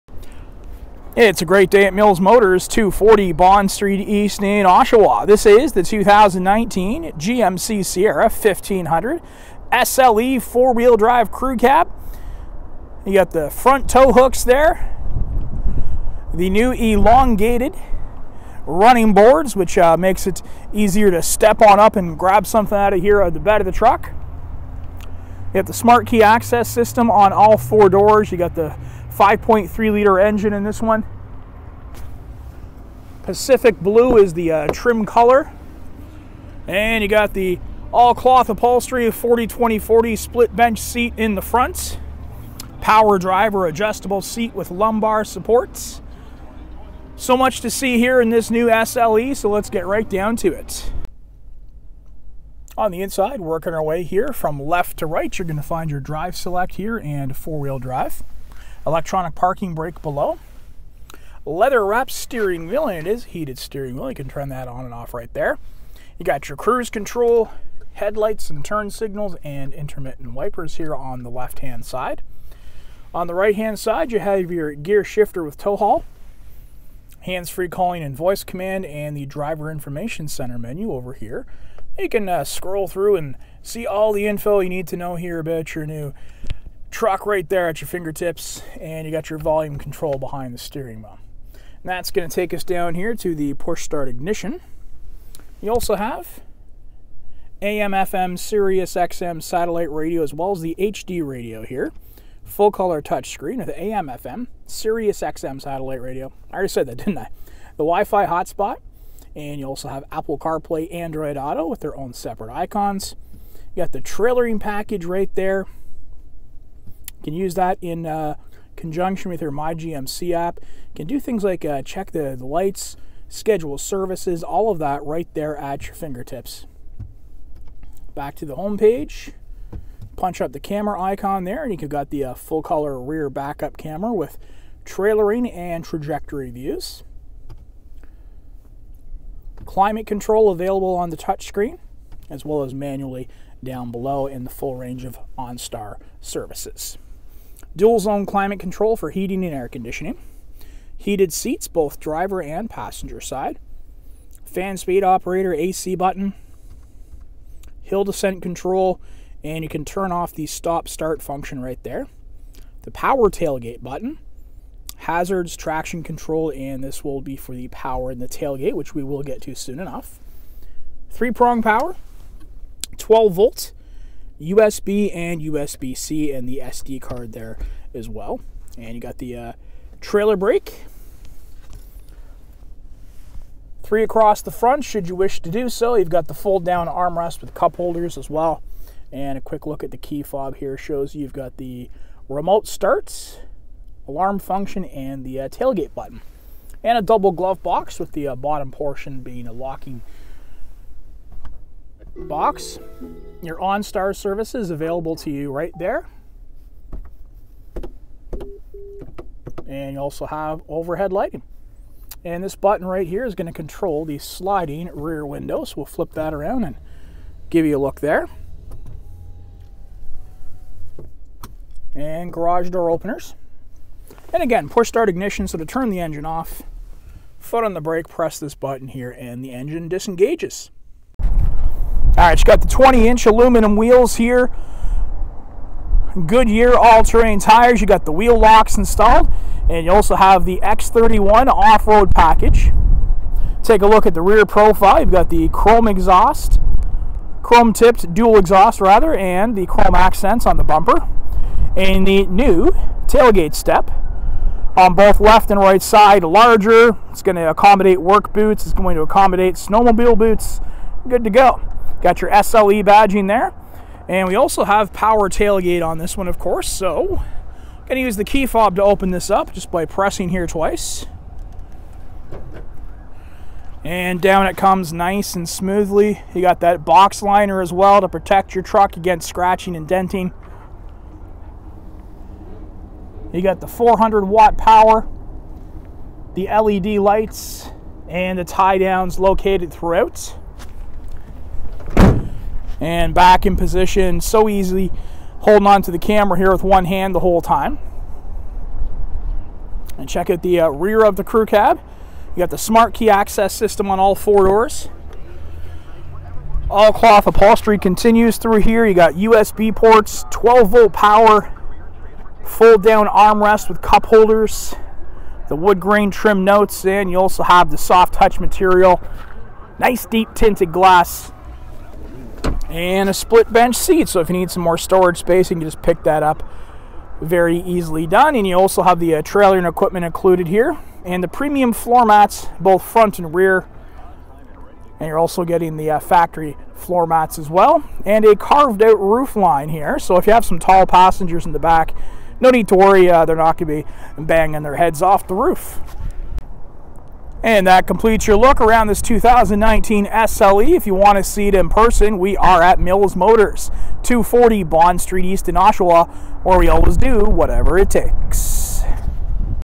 2019 GMC Sierra 1500 SLE Crew Cab Power Tailgate Blue Oshawa ON Stock #190675
This is a Pacific Blue Metallic 2019 GMC Sierra 1500 4WD Crew Cab 147 SLE Review Oshawa null - Mills Motors Bu with Automatic transmission Pacific Blue Metallic color and Jet Black interior color. (Uploaded by DataDriver).